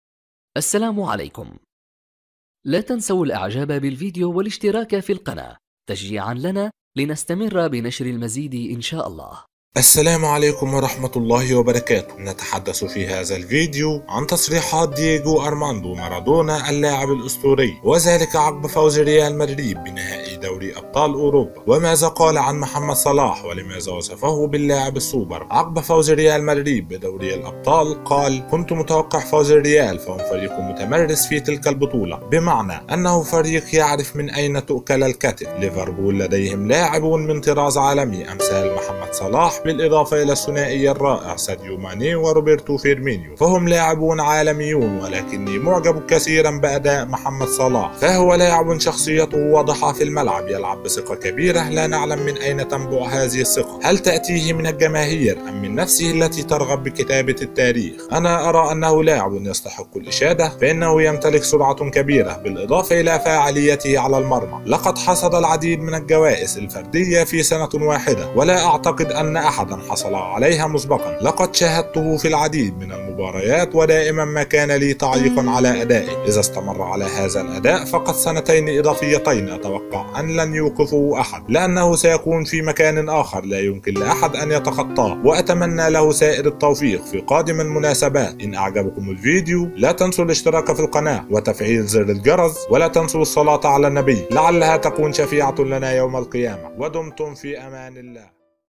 تصريح ماردونا عن محمد صلاح بعد مباراة ليفربول وريال مدريد بنهائي دوري ابطال اوروبا
السلام عليكم ورحمة الله وبركاته نتحدث في هذا الفيديو عن تصريحات ديجو ارماندو ماردونا اللاعب الاسطوري عقب فوز ريال مدريد بنهائي دوري ابطال اوروبا وماذا قال عن محمد صلاح ولماذا وصفة باللاعب السوبر
عقب فوز ريال مدريد بدوري الابطال قال كنت متوقع فوز الريال فهم فريق متمرس في تلك البطولة بالمعني يعرف من اين تاكل الكتف ليفربول لديهم لاعب من طراز عالمي وهو محمد صلاح بالاضافة الي الثنائي الرئع ساديو ماني و فيىمينيو فهم لاعبون عالميون ولكني معجب كثيرا باداء محمد صلاح فهو لاعب شخصيتة واضحة في الملعب يلعب بثقة كبير لا نعلم من اين تنبع هذة الثقة هل تاتية من الجماهير من نفسة التي ترغب بكتابة التاريخ انا اري انة لاعب يستحق الاشادة يمتلك سرعة كبير بالاضافة الي فعليتة علي المرمي لقد حصد جوائز فردية في سنة واحد لا اعتقد ان احد حصل عليها مسبقا لقد شاهدتة في العديد من المباريات ودائما ما كان لي تعليق علي ادئه اذا استمر علي هذا الاداء فقط سنتين اضافيتين اتوقع لن يوقفة احد لانة سيكون في مكان اخر لا يمكن لاحد ان يتخطاه واتمني لة سائر التوفيق في قادم المناسبات